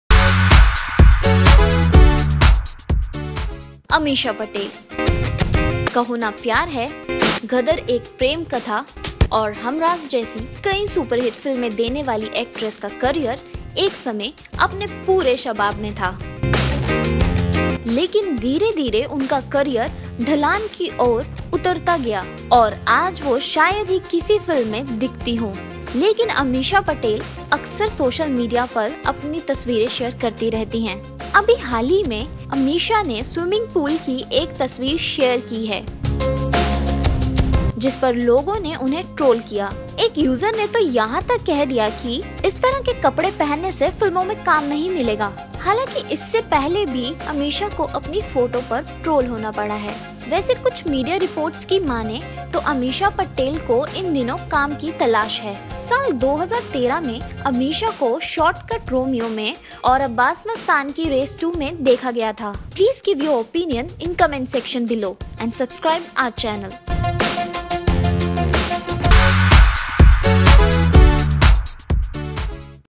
Ammesha Patel Trolled On Her Latest Photoshoot
Ameesha Patel has been trolled on her latest photo shoot in a swimming pool. Check Out a video